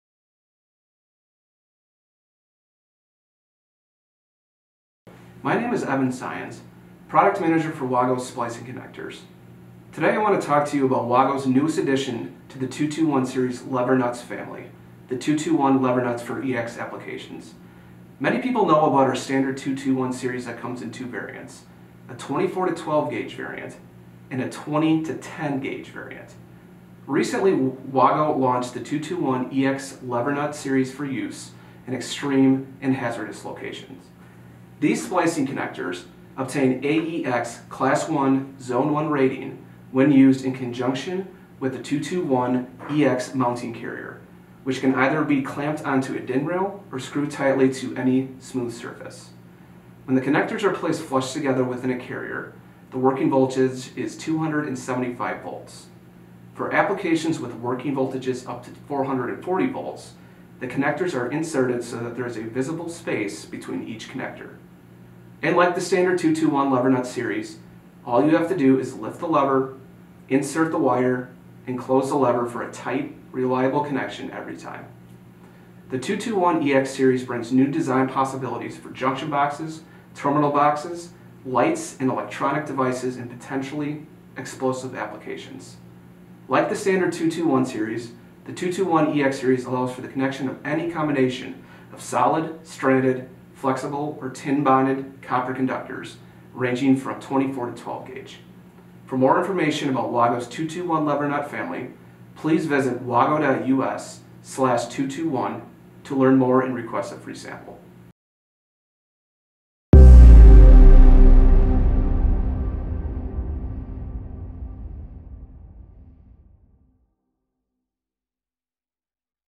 Product Manager Minute: 221 Ex Wire Splicing Connectors
Evan Syens, WAGO Product Manager for Electrical Splicing Connectors discusses the advantages of our new 221 wire splicing connectors for hazardous locations